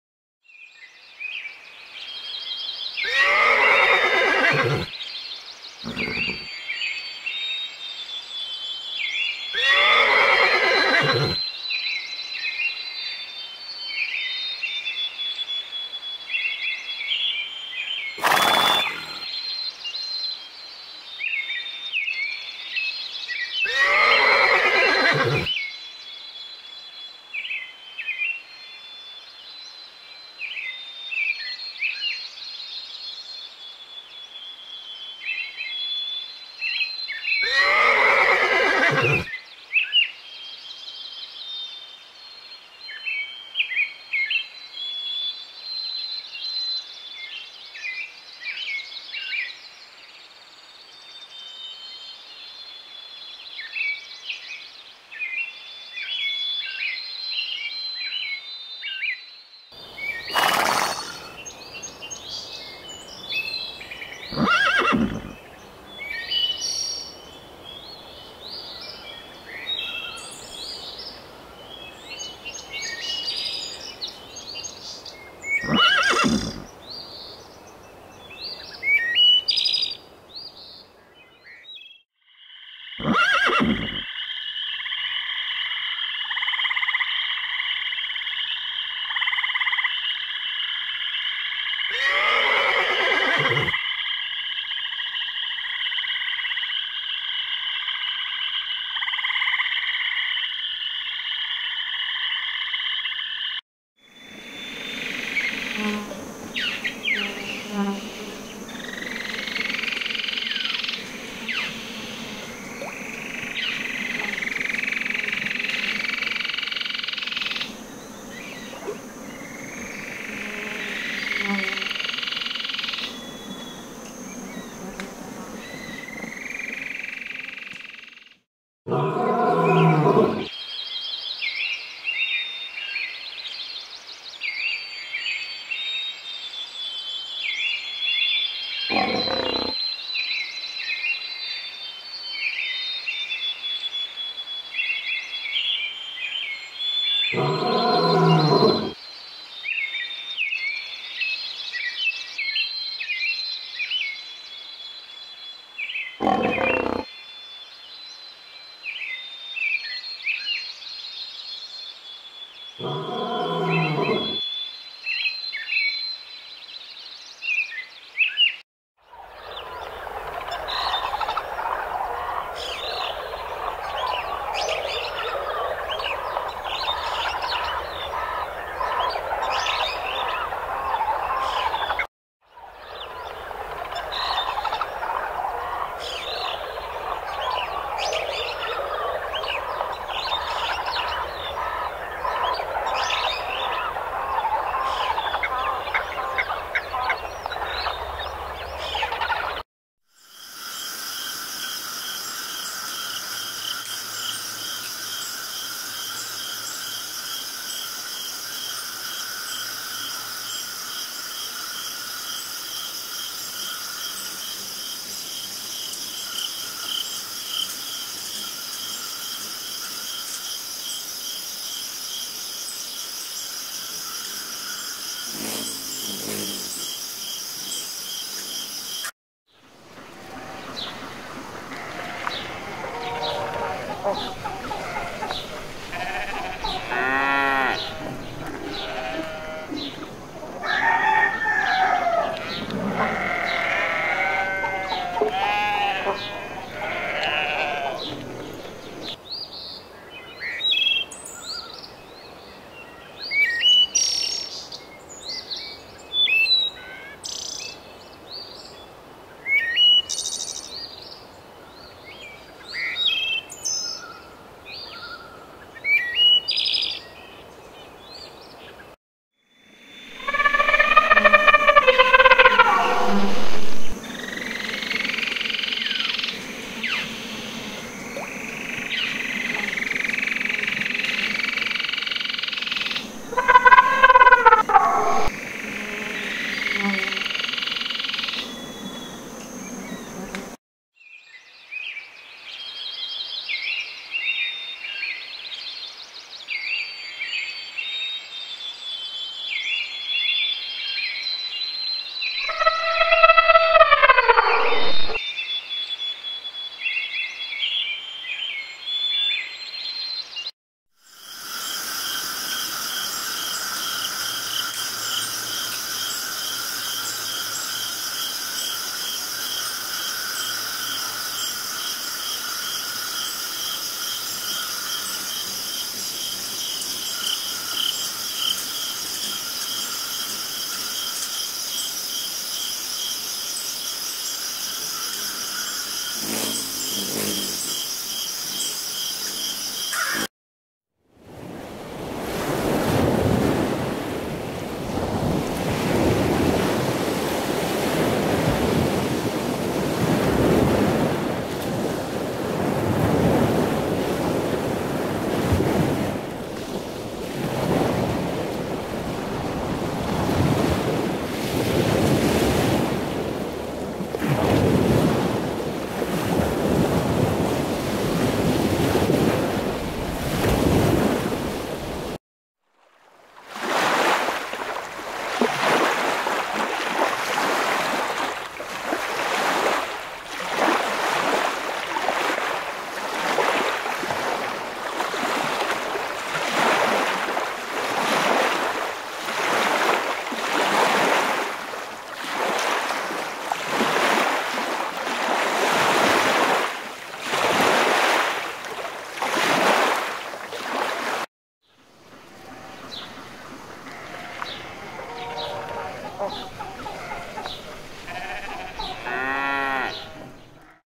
4K Amazing Scenes of Animal Wildlife with Natural Sound Effects - Relaxing Video
4K Amazing scenes of animal wildlife with natural sound effects. It's very relaxing and calming to watch.